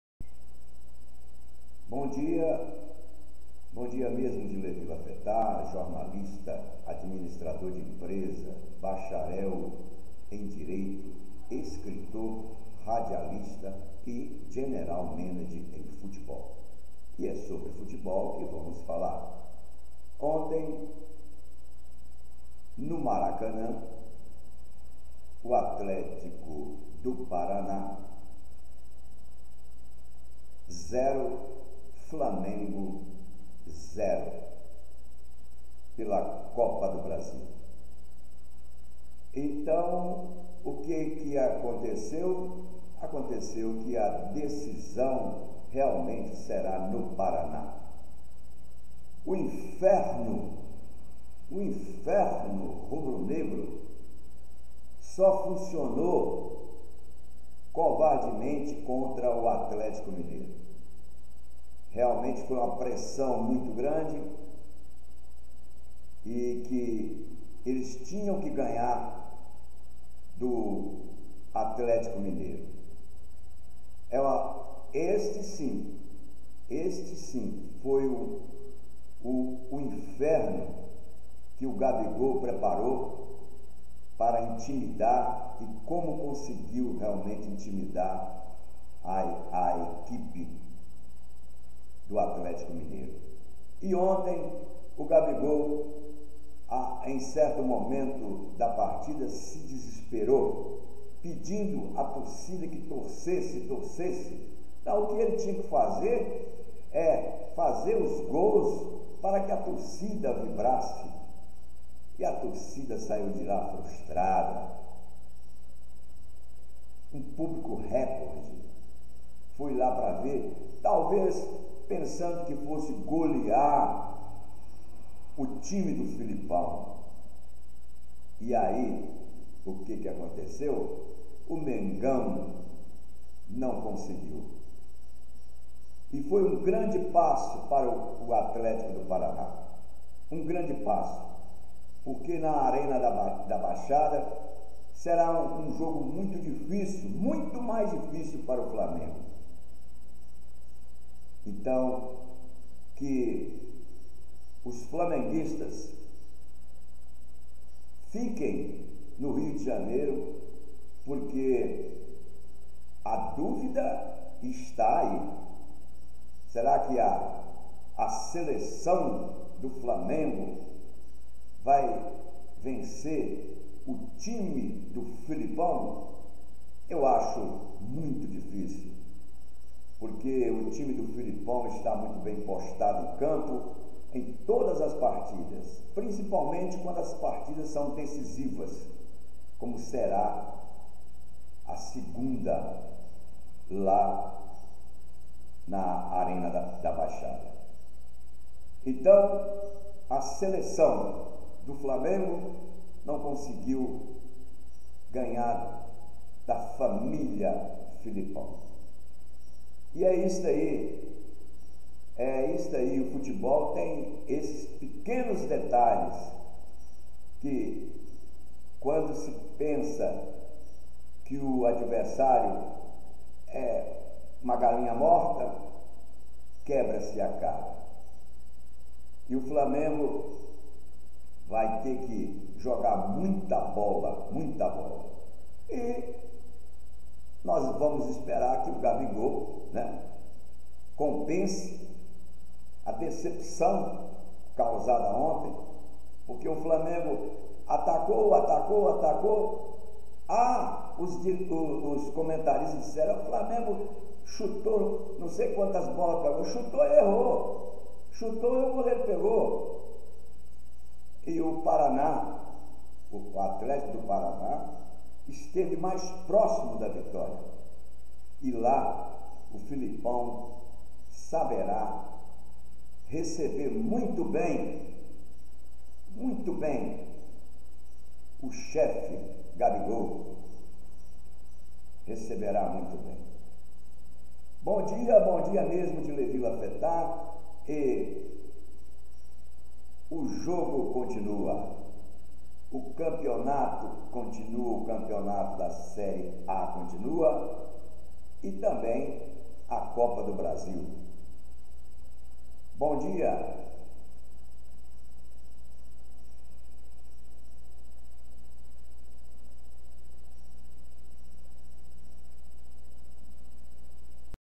O FURACÃO DE FELIPÃO PAROU O MENGÃO / GABIGOL NÃO ACENDEU O "INFERNO"! DECEPÇÃO !
Levi Lafeta - Jornalista, Radialista, Administrador de Empresa, Escritor, Bacharel em Direito e General Manager em futebol ( blog: bureau sportif lafeta )